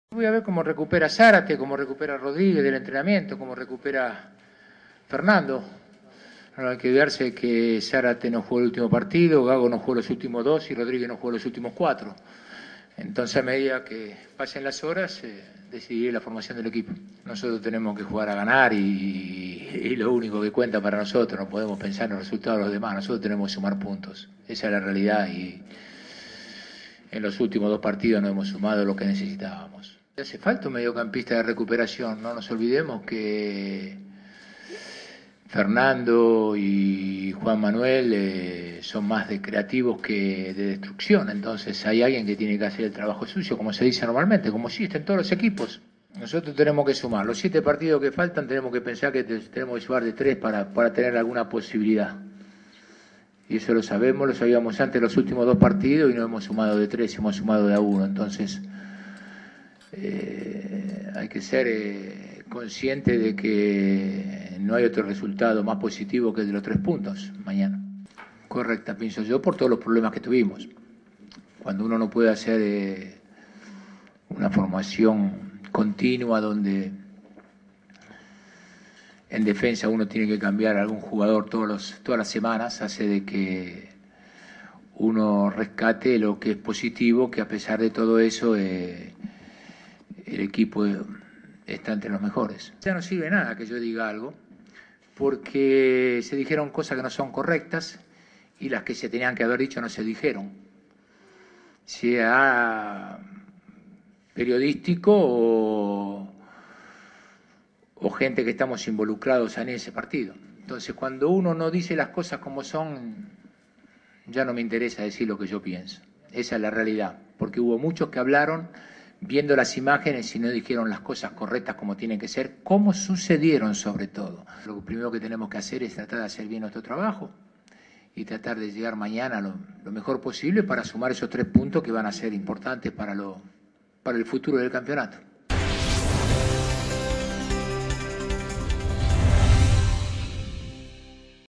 Bianchi: "Hasta ahora hicimos una campaña correcta"
Carlos Bianchi, DT de Boca Juniors, expresó hoy que su equipo hizo hasta el momento una
"campaña correcta" en el Torneo Inicial por "todos los inconvenientes" que tuvo, y aseguró que tendrán que "sumar de a tres" en los siete cotejos que quedan "para pelear por el título".
"Hasta ahora hicimos una campaña correcta por todos los inconvenientes, las lesiones, que tuvimos. No pudimos repetir un equipo de una fecha a otra y a pesar de esto el equipo está entre los mejores", expresó el DT de Boca, que mañana recibirá a Colón de
Santa Fe por la 13ra. fecha del Torneo Inicial.
Sobre la actualidad de Boca, que está cuarto a cinco puntos del líder Newell´s, el `Virrey´ consideró que no pueden "pensar en los demás", en referencia a los resultados de sus rivales en la puja por la corona, y aclaró: "Estamos obligados a sumar puntos y en los últimos dos partidos no sumamos lo que esperábamos".
"Hay que ser conscientes de que no hay otro resultado más positivo que los tres puntos en el partido de mañana", señaló sobre el encuentro ante Colón, rival que "viene un poco herido y va a pelear palmo a palmo".
"Tenemos que sumar de a tres en los siete partidos que quedan del campeonato para poder pelear con el título", añadió el entrenador, que luego se sinceró: "Ya lo sabíamos antes de estos dos últimos partidos y sólo sumamos uno en cada uno".
En otro orden, con respecto a los incidentes entre jugadores en el final del encuentro contra Godoy Cruz, en la fecha anterior, Bianchi se mostró molesto "con las cosas incorrectas que se dijeron" y no quiso dar su opinión.
"Me cuestionaron porque dije que no había visto nada. Yo estaba a 80 metros de lo que pasó. El martes vi todo lo que había sucedido pero ahora ya no sirve que yo diga algo. Se dijeron cosas que no son correctas. Ya no me interesa decir lo que pienso, no dijeron las cosas como sucedieron", declaró.
En tanto, para mañana, Bianchi no confirmó el equipo pero dio la lista de concentrados después de la práctica matutina en Casa Amarilla.
Los citados son Agustín Orión y Emanuel Trípodi (arqueros), Nahuel Zárate, Claudio Pérez, Daniel Díaz, Ribair Rodríguez, Guillermo Burdisso (defensores); Federico Bravo, Fernando Gago, Jesús Méndez, Juan Sánchez Miño, Juan Román Riquelme (volantes); Franco Cángele, Joel Acosta, Juan Manuel Martínez, Claudio Riaño, Emanuel Gigliotti y Nicolás Blandi (delanteros).
Boca, que suma 21 puntos, recibirá mañana a Colón, anteúltimo con 11, desde las 18.15, en el marco de la 13ra.fecha del Torneo Inicial.